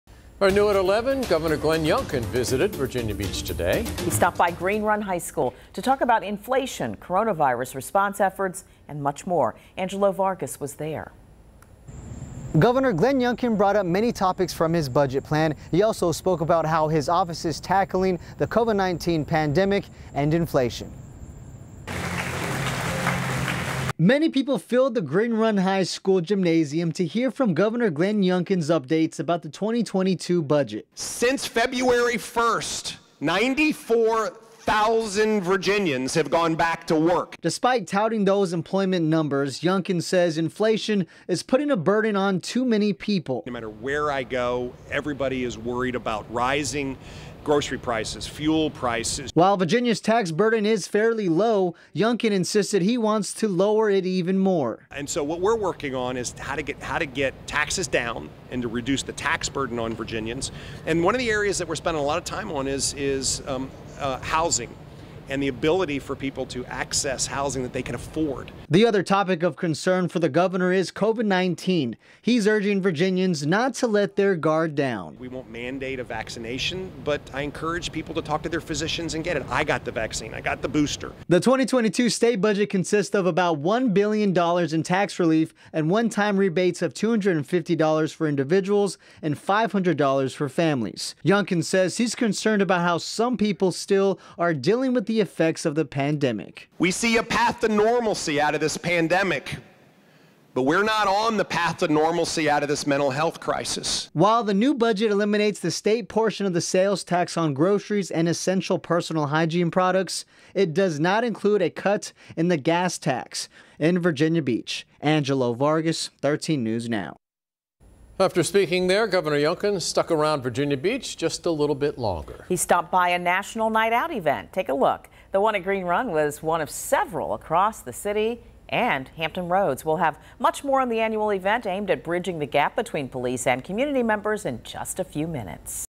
Youngkin discusses budget, COVID-19, and inflation in Virginia Beach
Gov. Glenn Youngkin visited Virginia Beach to provide updates to his 2022 budget plan.